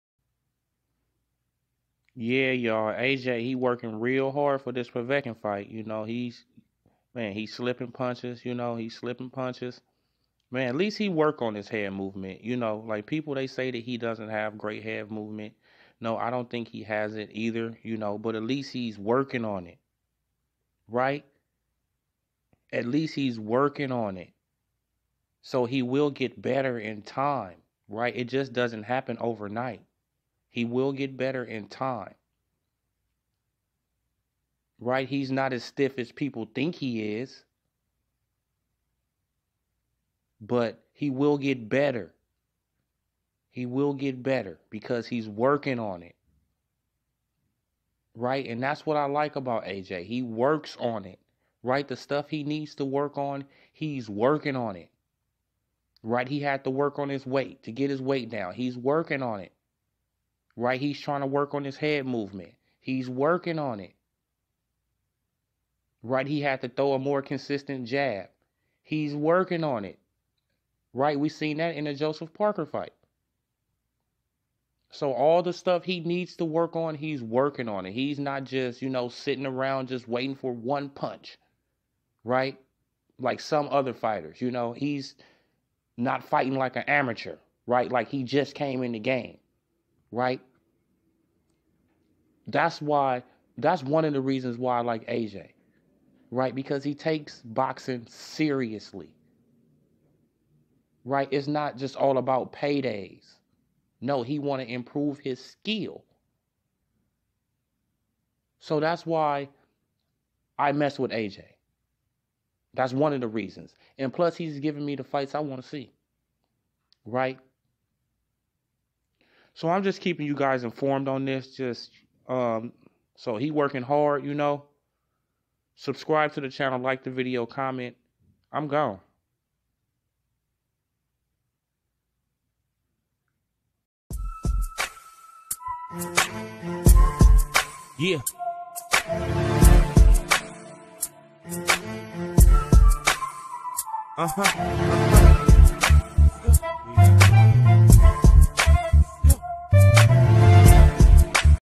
Joshua Working On Slipping Punches And Countering..
Odee Locc Sports Info Let's Go...